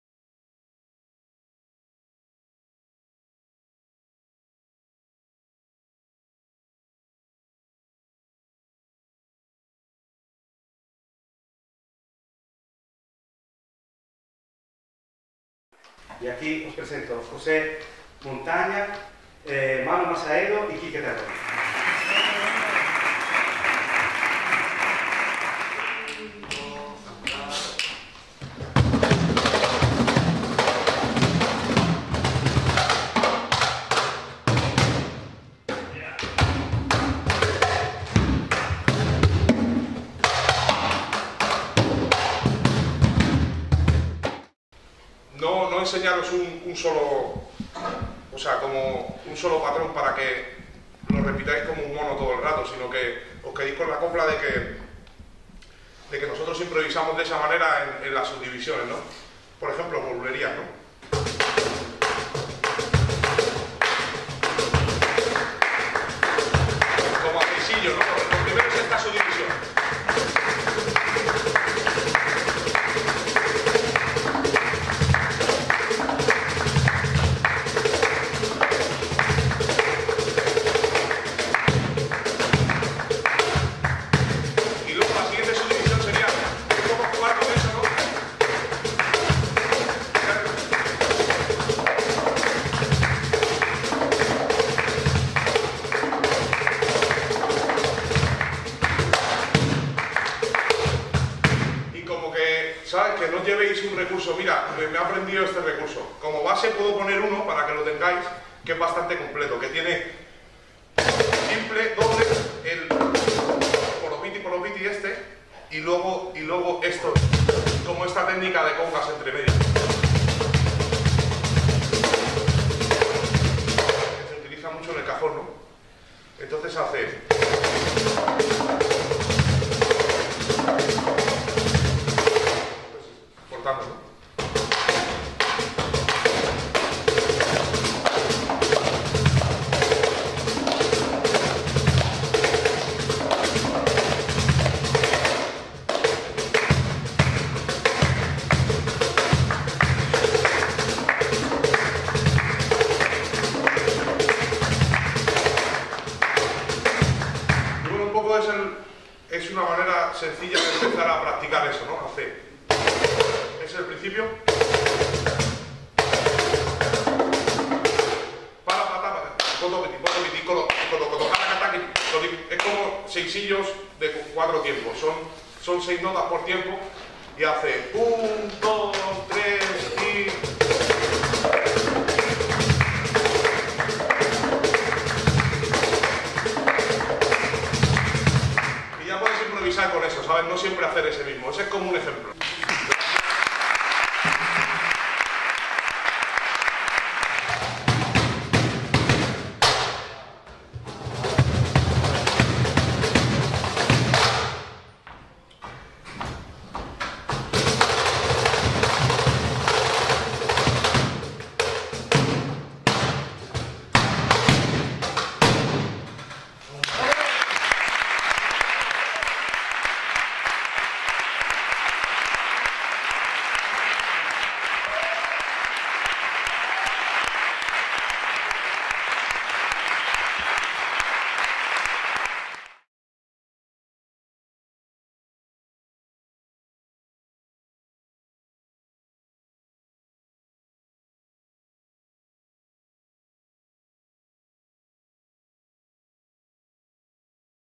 Masterclass de cajón de José Montaña 2016
Grabado en el XIV Seminario Internacionál de Cajón de Sant Boi - Barcelona, con Manu Masaedo y Kike Terrón.
Recorded at the 14th International Cajón Seminar in Sant Boi - Barcelona with Manu Masaedo and Kike Terrón.